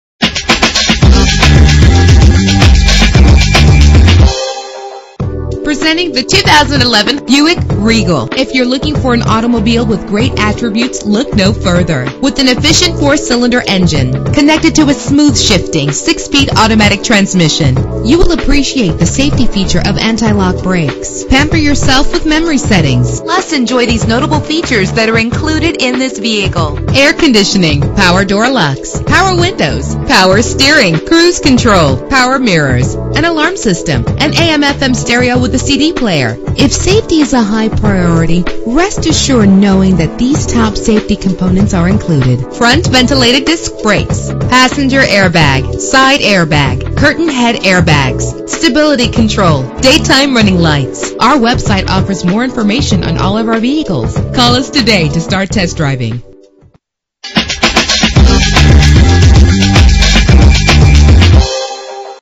2011 Buick Regal - Warren OH
Year: 2011
Make: Buick
Model: Regal
Trim: CXL
Engine: 2.4 liter inline 4 cylinder 16 valve
Transmission: 6-Speed Automatic
Color: Silver
Mileage: 0
Address:
3100 Elm Road North East